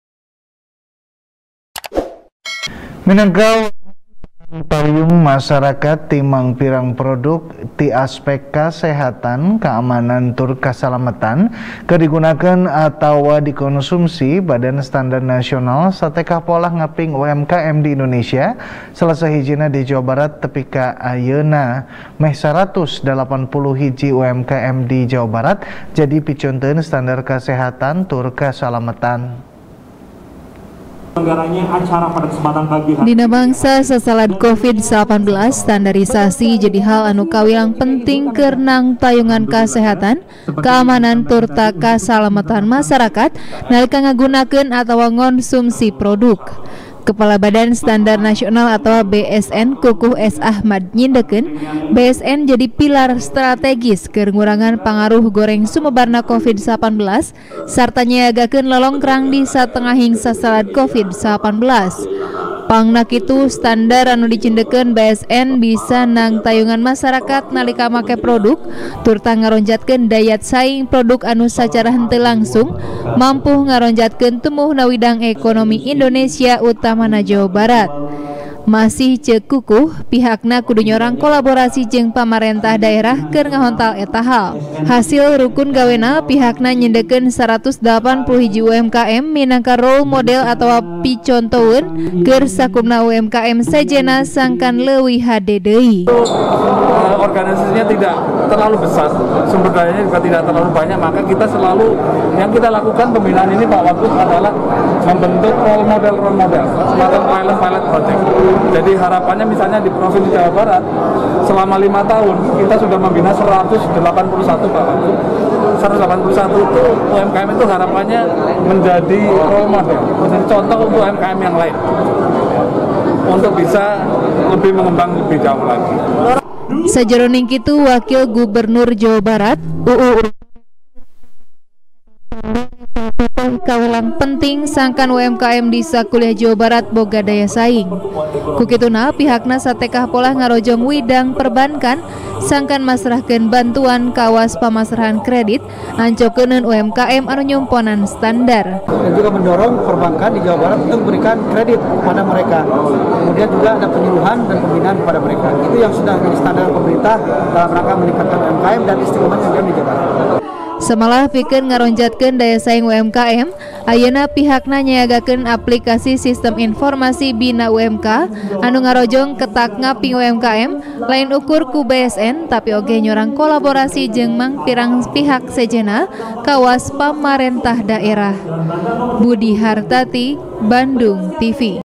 181 UMKM Jadi Role Model SNI | TP | BANDUNGTV
TP | BANDUNGTV | DERI FIRMAN | Minangka Wangun Panangtayung Masarakat Ti Mangpirang Produk Tina Aspek Kasehatan, Kaamanan, Tur Kasalametan Keur Digunakeun Atawa Dikonsumsi, Badan Standar Nasional Satekah Polah Ngaping UMKM Di Indonesia, Salasahijina Jawa Barat. Tepi Ka Ayeuna, Meh Saratus Dalapan Puluh Hiji UMKM Di Jawa Barat Jadi Picontoneun Standar Kasehatan Tur Kasalametan.